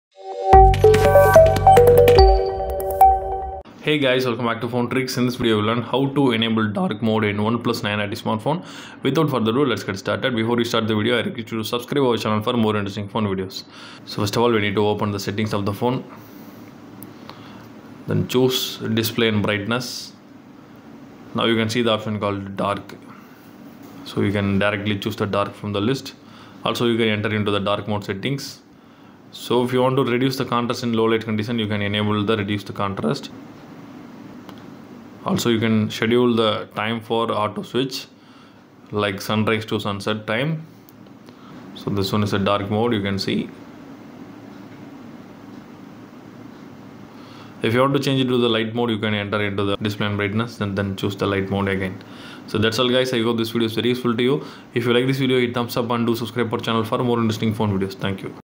How to enable Darkmode in Oneplus 9RT
Hey Friends  ! Checkout this new video of How to enable Darkmode in Oneplus 9RT